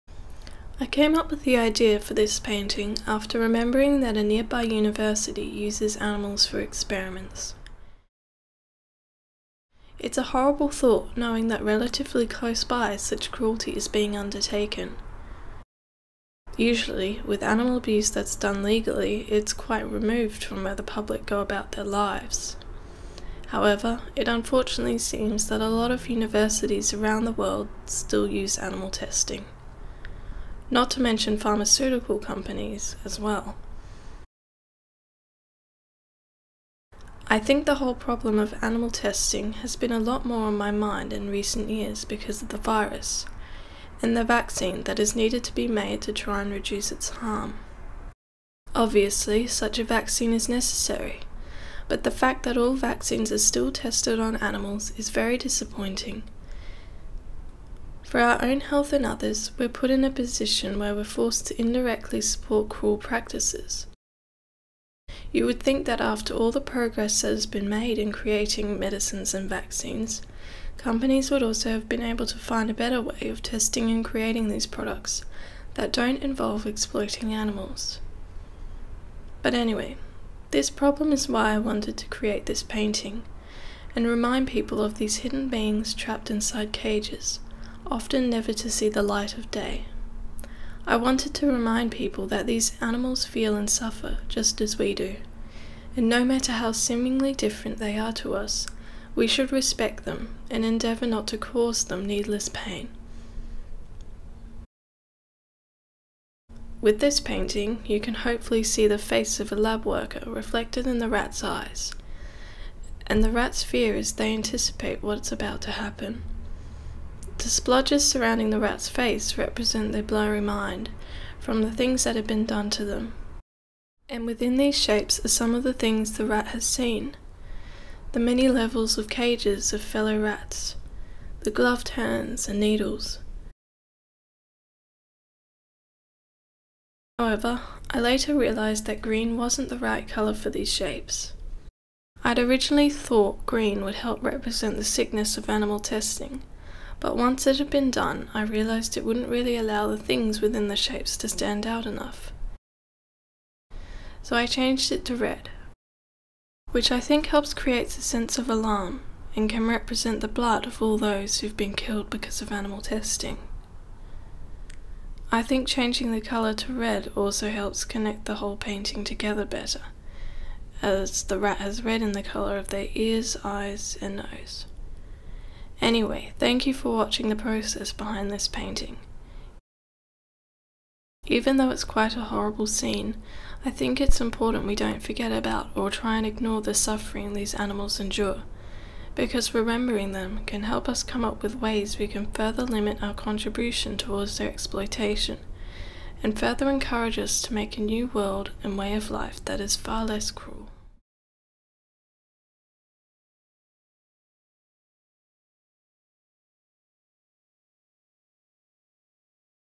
Acrylic painting of a lab rat | Timelapse
This is a timelapse of my most recent painting using acrylics. It was created to bring attention to the cruelty of animal testing, and the importance that we progress beyond needless animal exploitation.